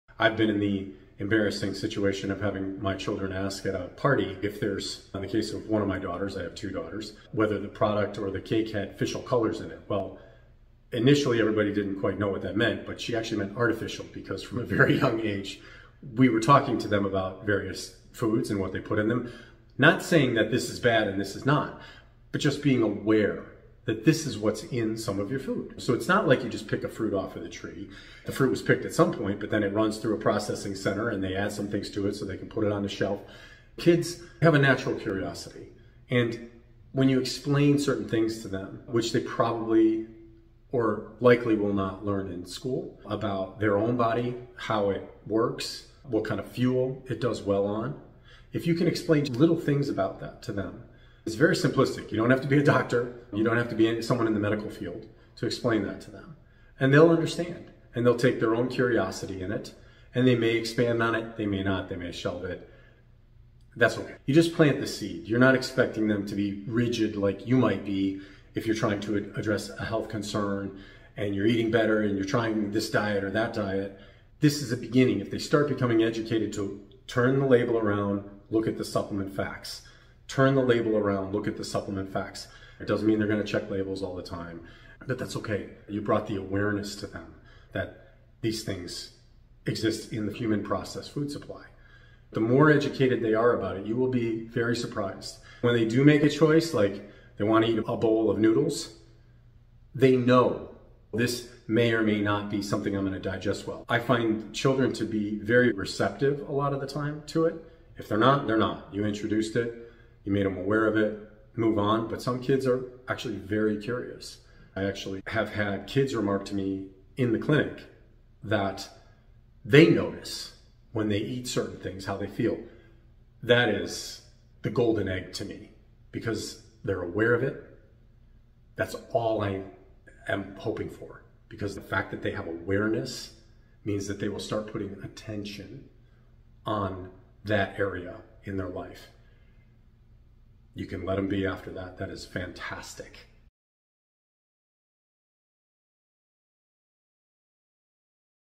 Why it's important to teach kids to read food labels.
This is a video about why it's important to teach kids to be aware of food labels.
Not only will this help them make informed decisions about what they eat, but it will also help them with their health and Nutrition education!

Not all food is created equal and not all food has the same ingredients. That's why it's so important for kids to be aware of what's going into their bodies by what they're eating. By teaching them to read food labels, we can help them make informed decisions and improve their overall health!

This is NOT a video about the individual ingredients listed on food labels.